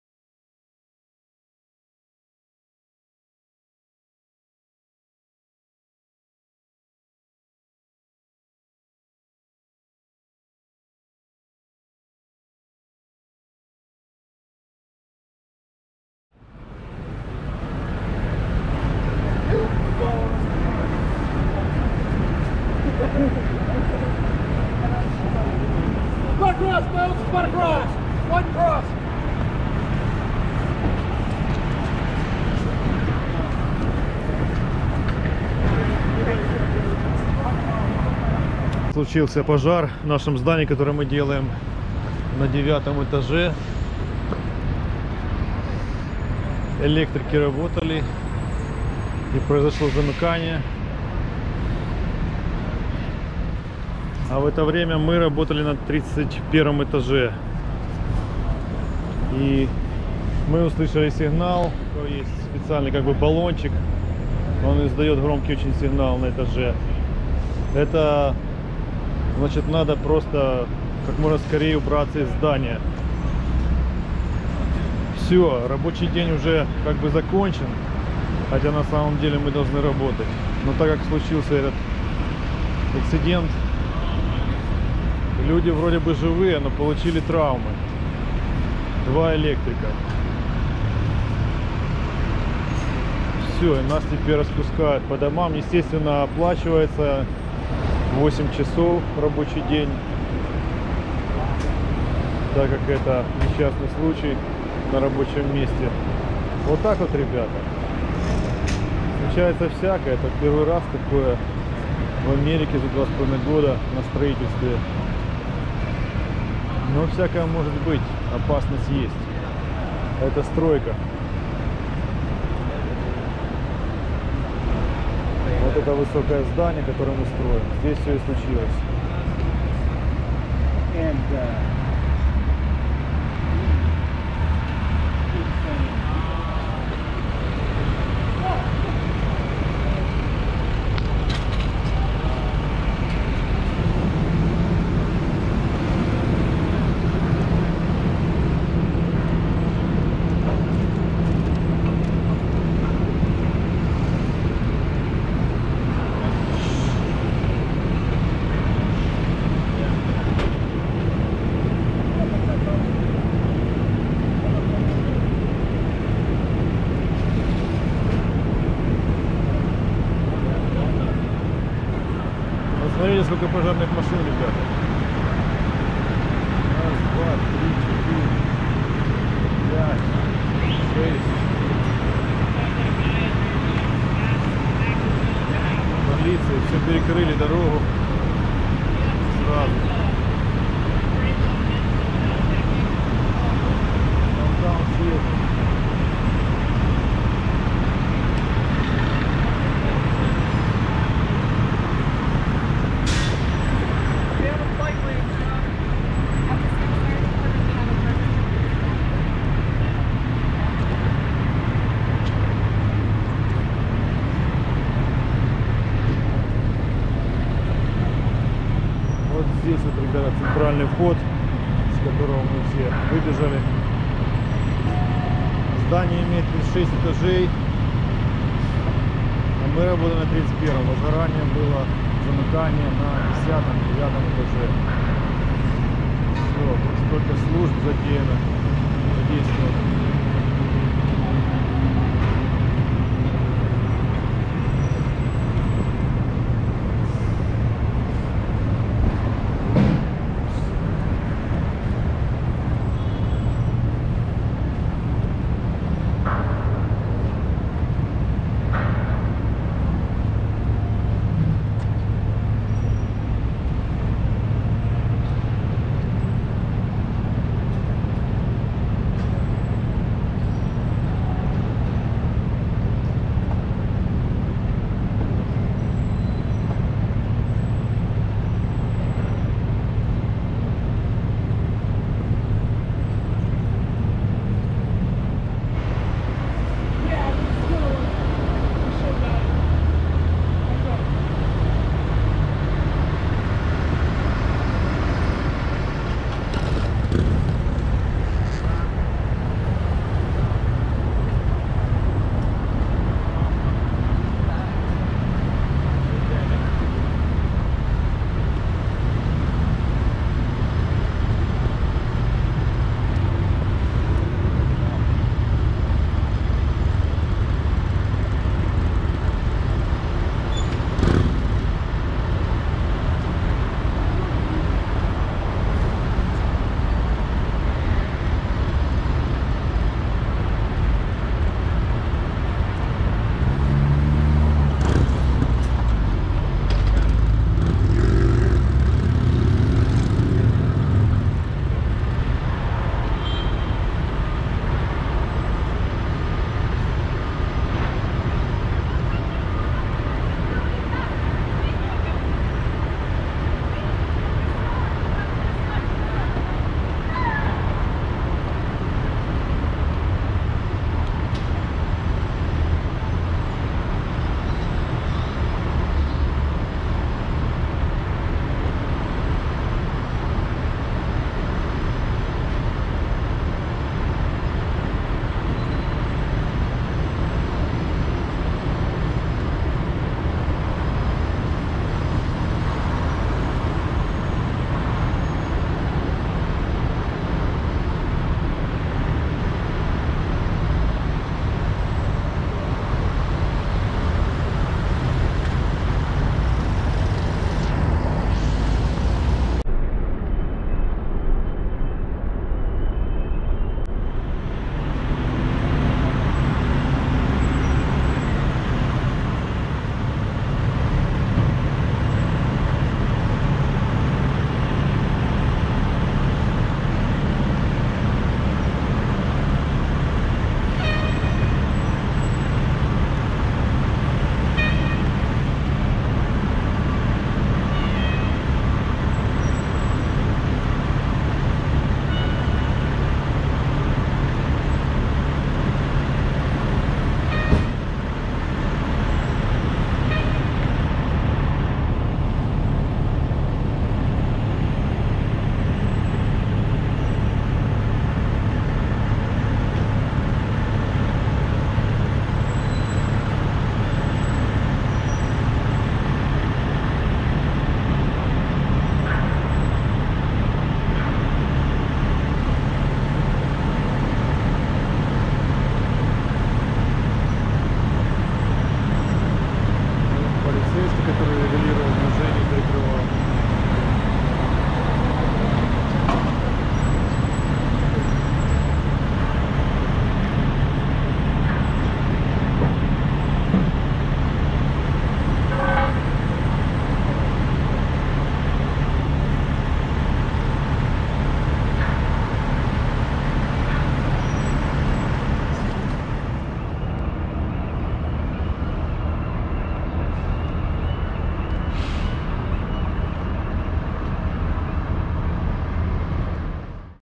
ВЗРЫВ И ПОЖАР ВЫСОТКИ В ЦЕНТРЕ СИЭТЛА, КОТОРУЮ МЫ СТРОИМ!УДАЛОСЬ СПАСТИСЬ, НО ЕСТЬ ЖЕРТВЫ!
Сегодня произошел взрыв и возгарание на 9-ом этаже здания, которое мы строим.Пришлось бросать всё и бежать! Удалось благополучно спуститься с 31-го этажа, но люди пострадали!